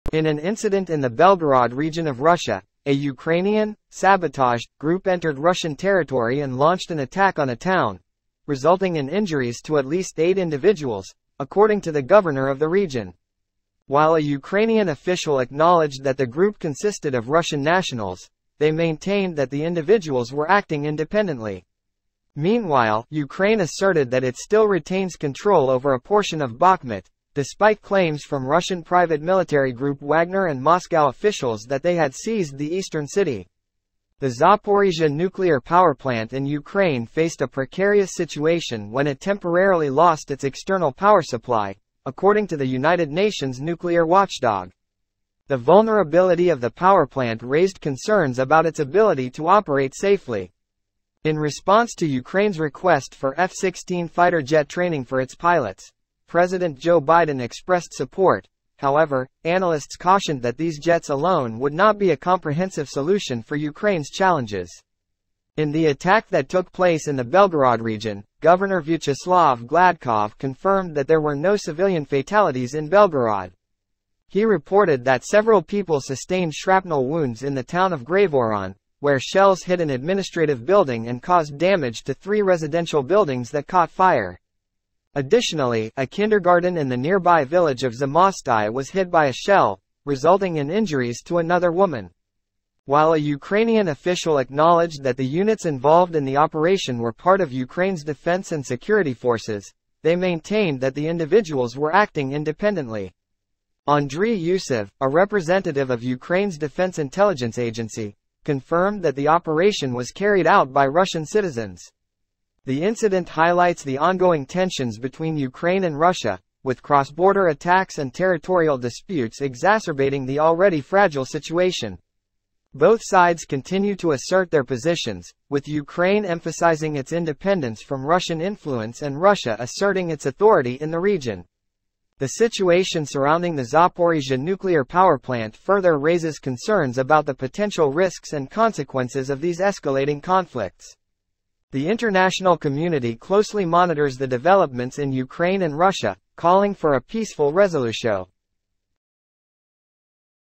Tensions escalate in Bakhmut and Russia-Ukraine conflict.
In an incident in the Belgorod region of Russia, a Ukrainian "sabotage" group entered Russian territory and launched an attack on a town, resulting in injuries to at least eight individuals, according to the governor of the region. While a Ukrainian official acknowledged that the group consisted of Russian nationals, they maintained that the individuals were acting independently.
Meanwhile, Ukraine asserted that it still retains control over a portion of Bakhmut, despite claims from Russian private military group Wagner and Moscow officials that they had seized the eastern city.
The Zaporizhzhia nuclear power plant in Ukraine faced a precarious situation when it temporarily lost its external power supply, according to the United Nations nuclear watchdog. The vulnerability of the power plant raised concerns about its ability to operate safely.
In response to Ukraine's request for F-16 fighter jet training for its pilots, President Joe Biden expressed support; however, analysts cautioned that these jets alone would not be a comprehensive solution for Ukraine's challenges.
In the attack that took place in the Belgorod region, Governor Vyacheslav Gladkov confirmed that there were no civilian fatalities in Belgorod. He reported that several people sustained shrapnel wounds in the town of Graivoron, where shells hit an administrative building and caused damage to three residential buildings that caught fire. Additionally, a kindergarten in the nearby village of Zamostye was hit by a shell, resulting in injuries to another woman.
While a Ukrainian official acknowledged that the units involved in the operation were part of Ukraine's defense and security forces, they maintained that the individuals were acting independently. Andriy Yusov, a representative of Ukraine's defense intelligence agency, confirmed that the operation was carried out by Russian citizens.
The incident highlights the ongoing tensions between Ukraine and Russia, with cross-border attacks and territorial disputes exacerbating the already fragile situation. Both sides continue to assert their positions, with Ukraine emphasizing its independence from Russian influence and Russia asserting its authority in the region. The situation surrounding the Zaporizhzhia nuclear power plant further raises concerns about the potential risks and consequences of these escalating conflicts. The international community closely monitors the developments in Ukraine and Russia, calling for a peaceful resolution.